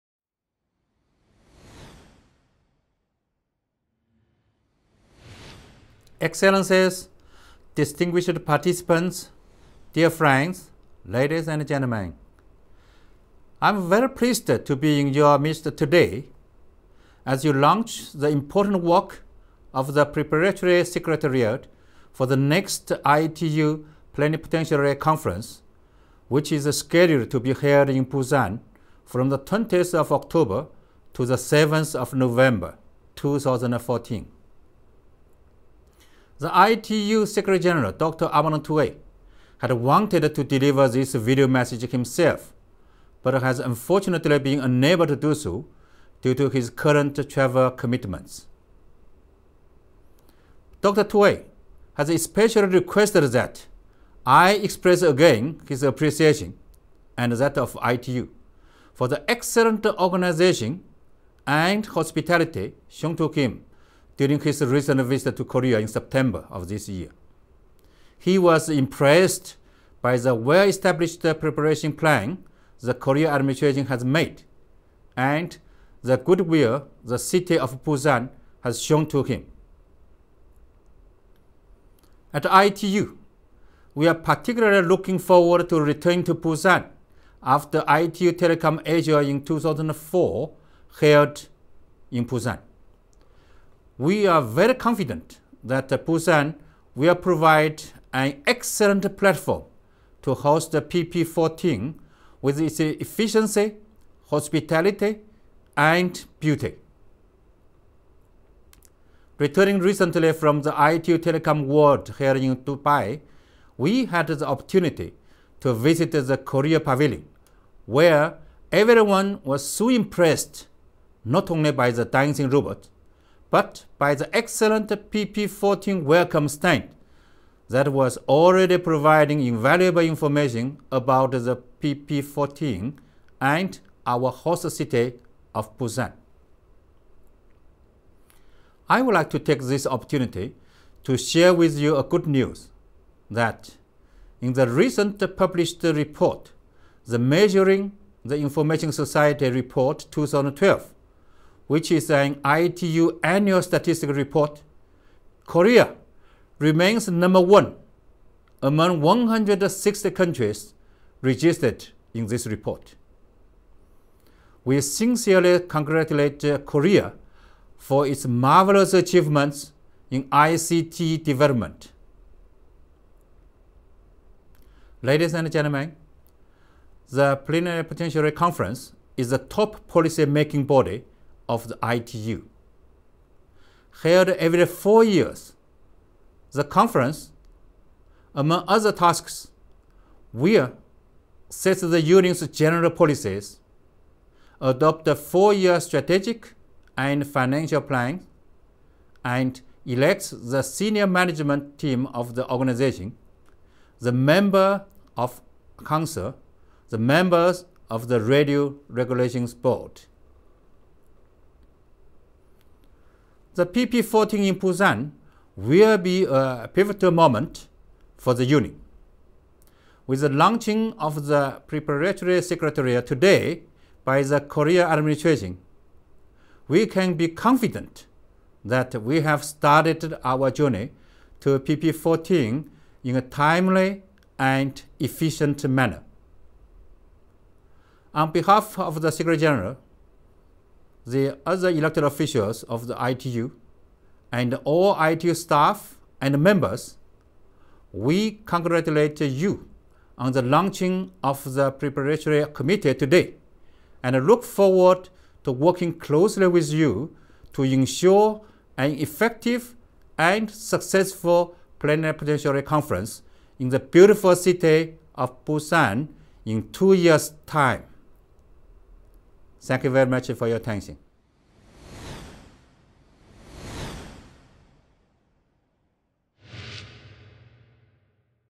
ITU Video Message: Mr Houlin Zhao, Deputy Secretary- General, ITU on the Launch of ITU PP-14
ITU Video Message: Mr Houlin Zhao, Deputy Secretary - General, ITU giving a video message on the launch of the ITU 2014 Plenipotentiary meeting (PP-14) which will be held in Busan, South Korea in 2014.